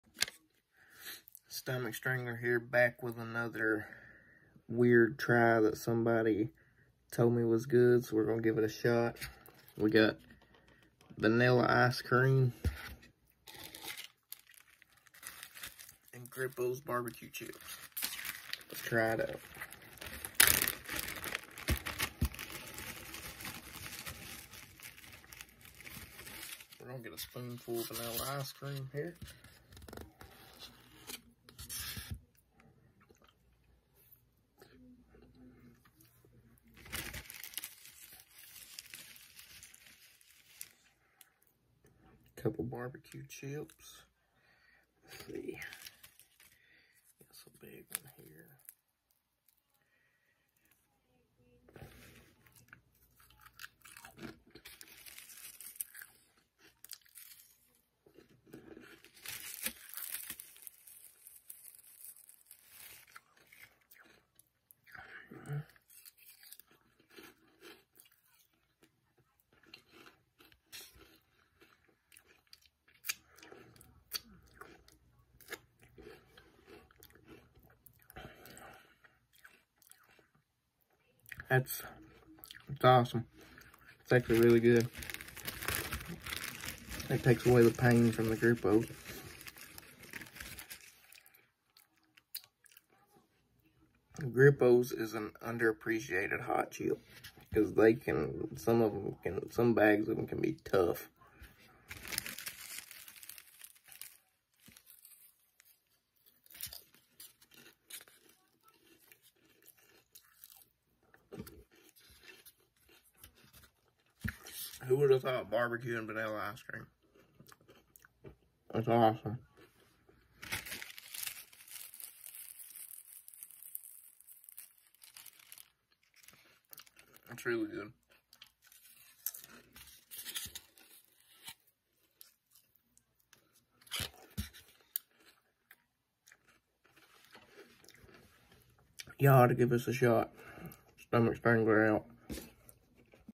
Weird Foods bbq/ice cream
Trying people fav weird snacks from Facebook